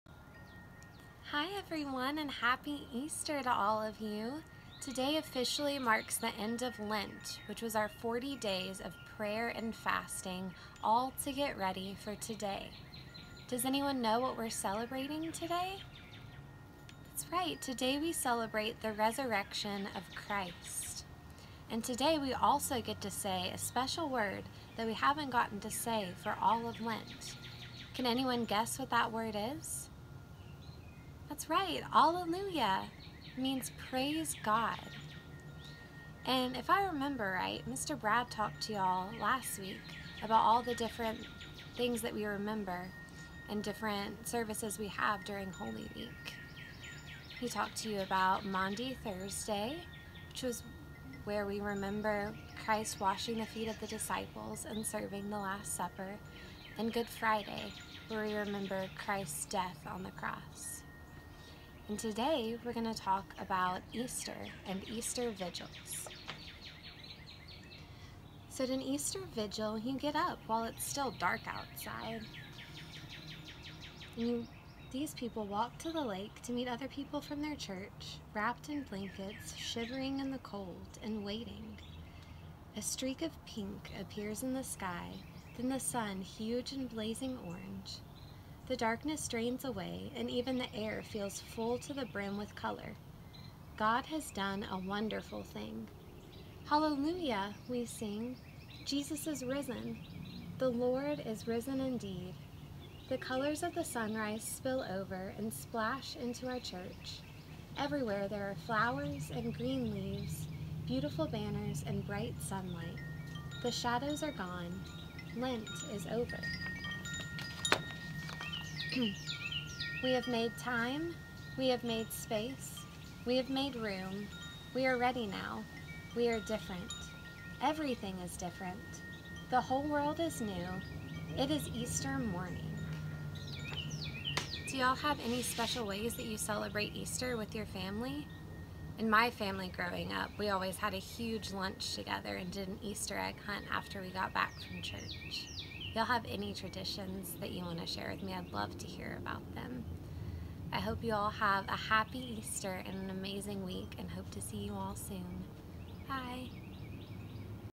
Mission Kids - Easter (April 12) Elementary Lesson
Intended for children ages 7-11 approximately. Families may choose to watch both or either April 12 video, dependent on family and children needs.

ALLELUIA! Christ is risen! Christ is risen indeed!